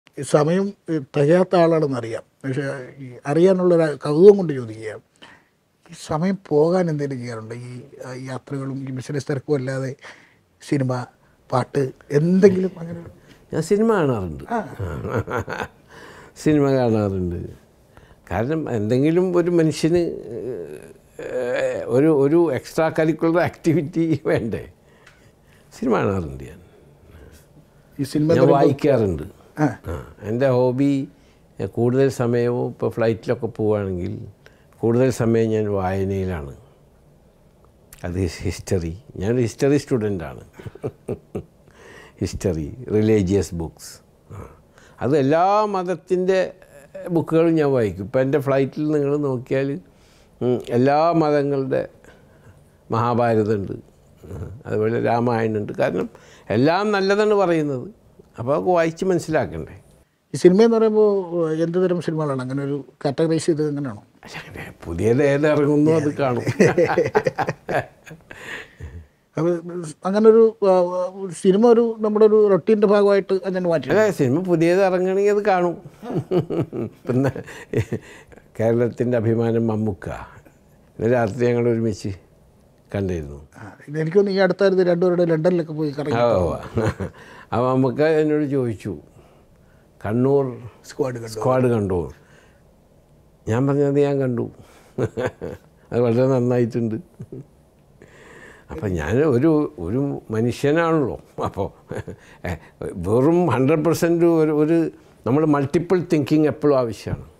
"Kannur Squad കണ്ടോയെന്ന് Actor Mammootty ചോദിച്ചു"; യൂസഫലി പറയുന്നു | MA Yusuff Ali Interview
MA Yusuff Ali Interview : ബിസിനസ് വിജയത്തെക്കുറിച്ച് പറഞ്ഞ് MA Yusuff Ali. News18 Keralaയ്ക്ക് അനുവദിച്ച അഭിമുഖത്താലാണ് Yusuff Aliയുടെ തുറന്ന് പറച്ചിൽ.